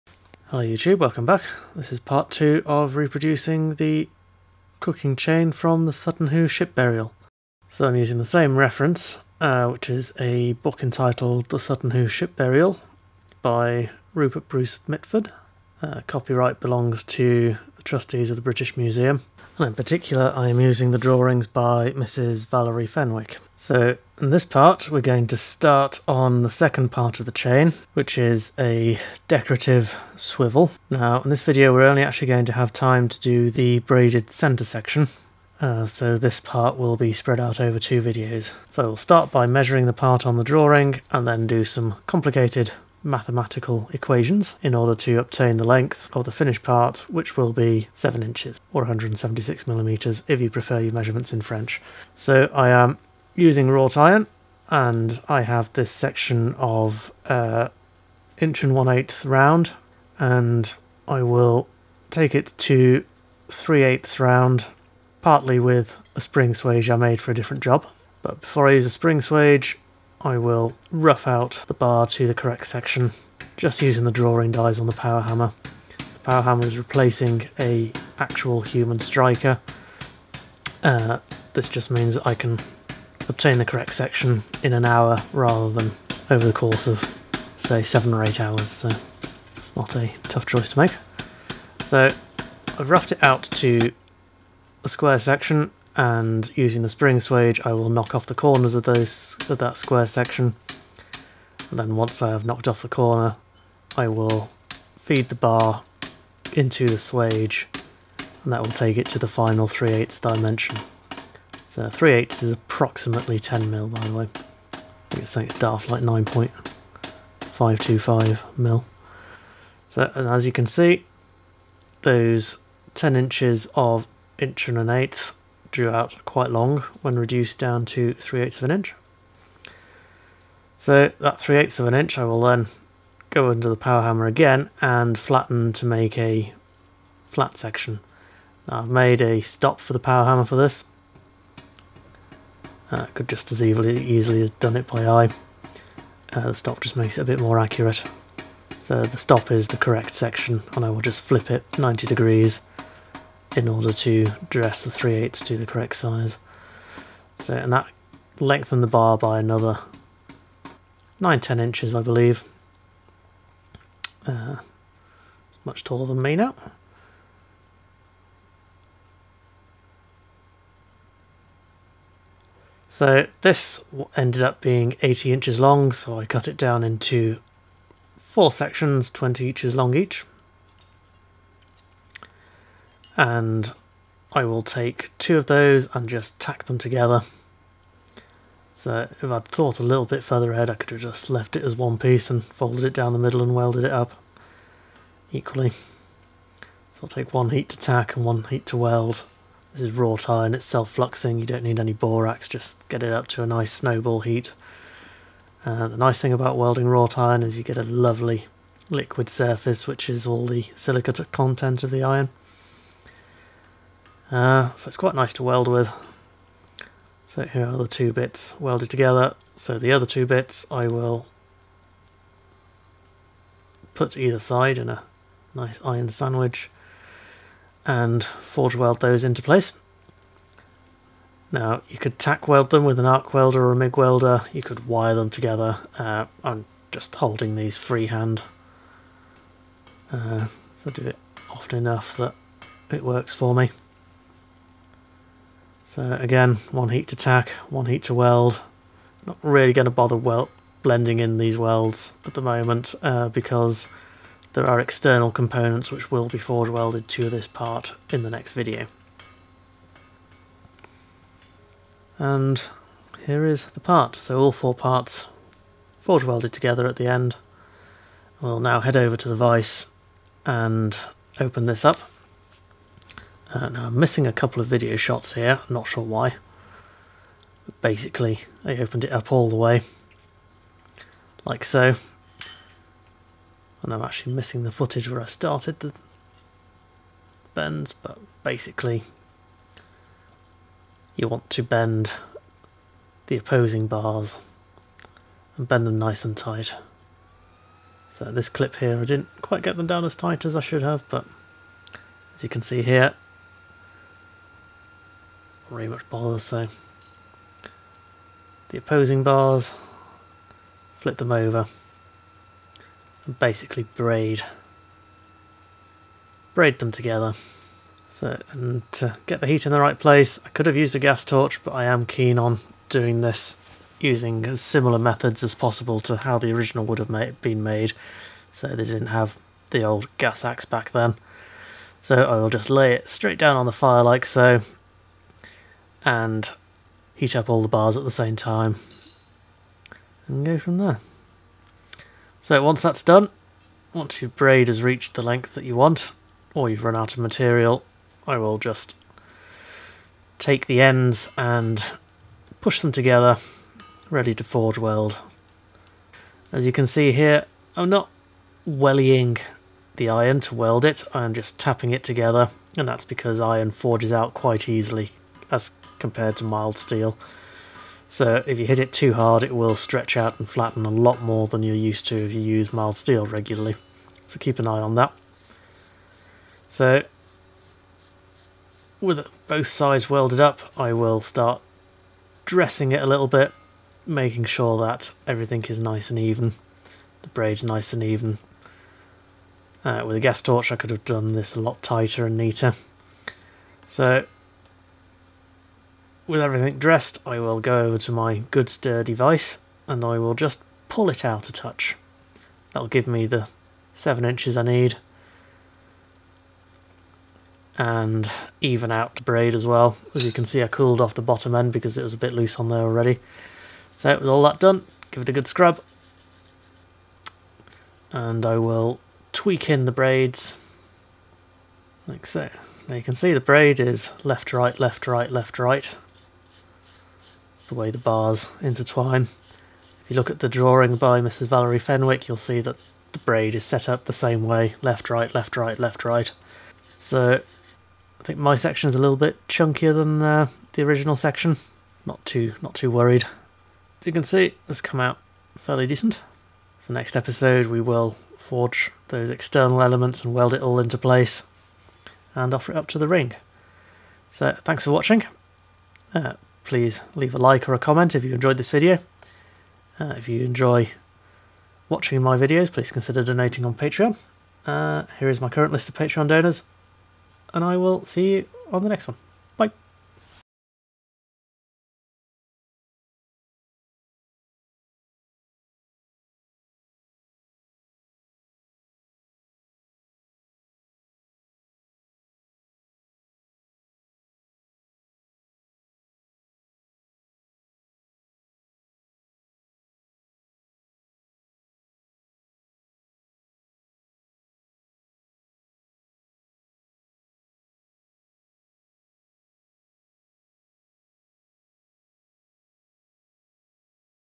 Forging the Sutton Hoo chain. Part 2, starting the swivel.
In this video, I start creating the decorative swivel for the chain. This involves drawing-out wrought-iron in order to create the braided central element of the swivel.